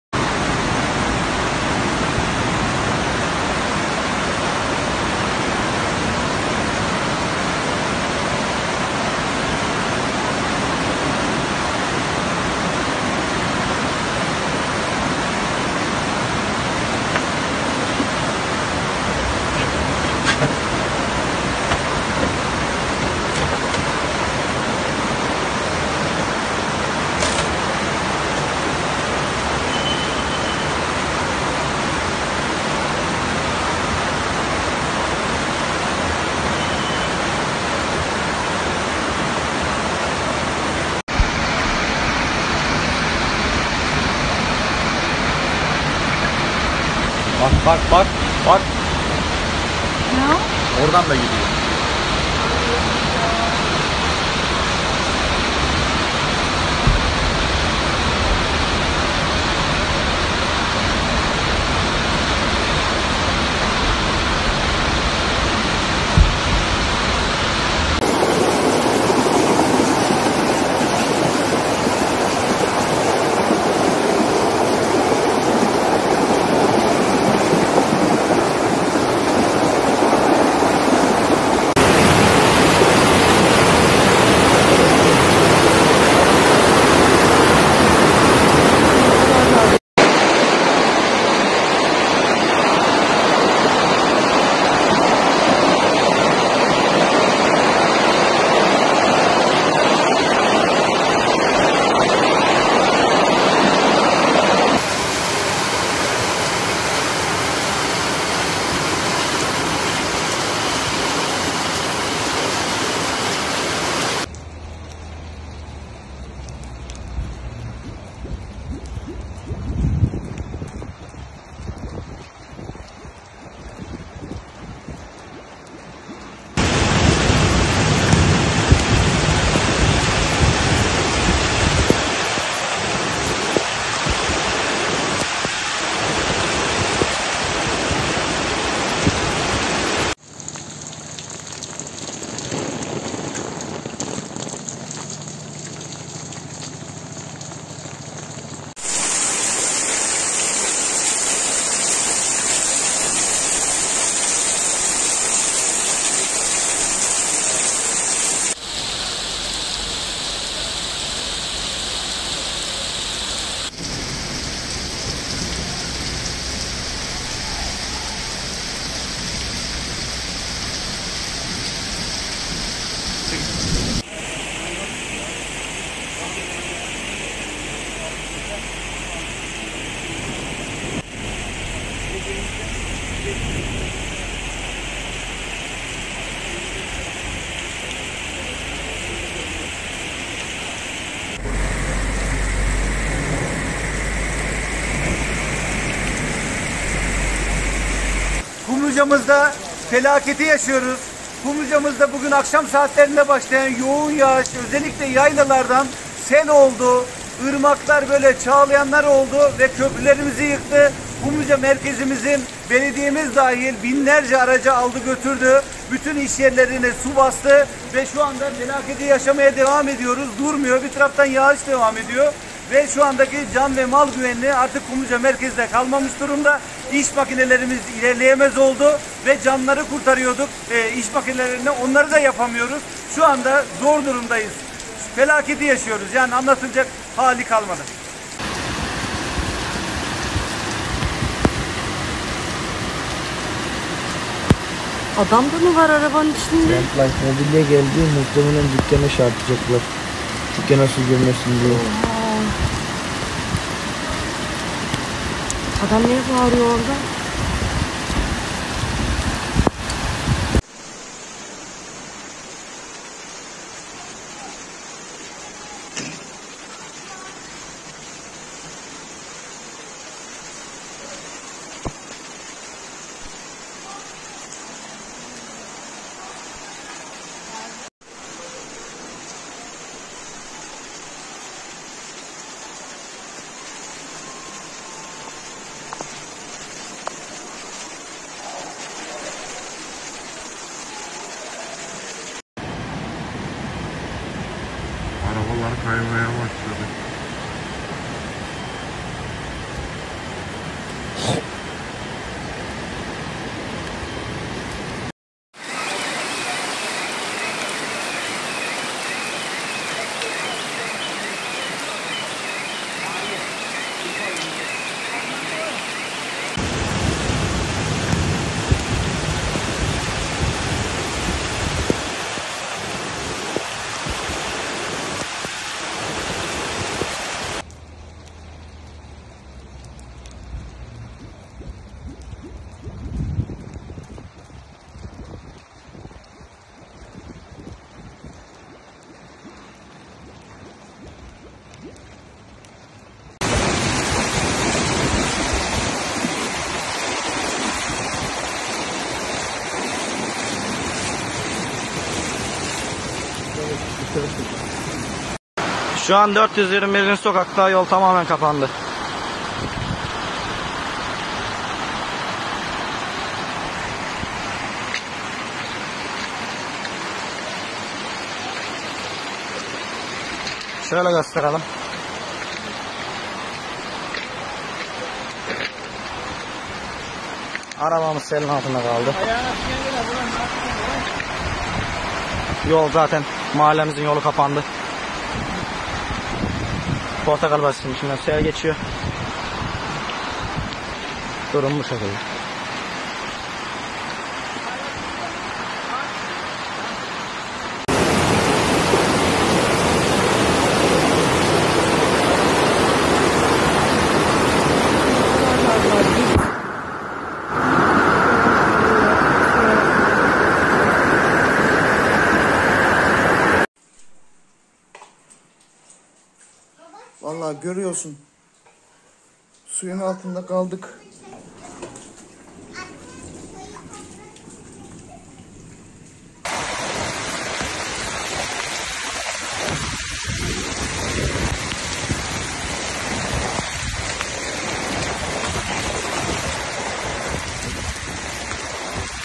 Antalya'da sel felaketi! Otomobiller sürüklendi, yollar göle döndü
Antalya'nın Kumluca ve Finike ilçelerinde akşam saatlerinde etkisini artıran yağmur, gece sel felaketine dönüştü. Taşan derelerden akan sel suları ile park halindeki otomobiller sürüklenirken, her iki ilçede çok sayıda ev ve iş yerinin zemin ve bodrum katlarını su bastı. AFAD ekiplerinden destek istenirken, yıkılan köprüler nedeniyle iş makinelerinin çalışamadığı belirtildi. Kumluca ve Finike'de ilk ve orta dereceli okullara 1 gün süreyle ara verildiğini duyurdu.